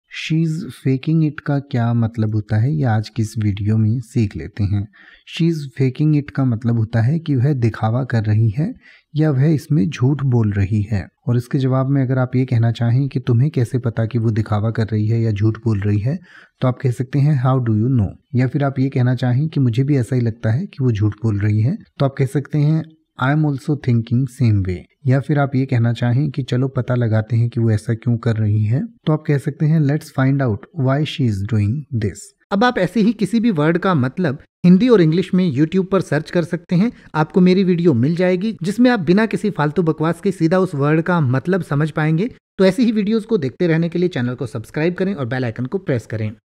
She is faking it meaning in Hindi | She is faking it ka matlab kya hota hai | English to hindi
इस वीडियो में आप She is faking it का हिंदी में मतलब समझेंगे और साथ में She is faking it का pronunciation भी सीखेंगें यानि की आप She is faking it के अर्थ के साथ साथ ये भी सीखेंगें की She is faking it को कैसे बोले या She is faking it को बोलने का सही तरीका क्या है |

तो बस एक मिनट में सीखिए She is faking it को|
Let’s learn Hindi Meaning of She is faking it in detail.

She is faking it का हिंदी में अर्थ होता है
वह यह दिखावा कर रही है

I am also thinking same way
Let's find out why she is doing this

She is faking it को इन उदाहरणों से और अच्छे से समझ सकते हैं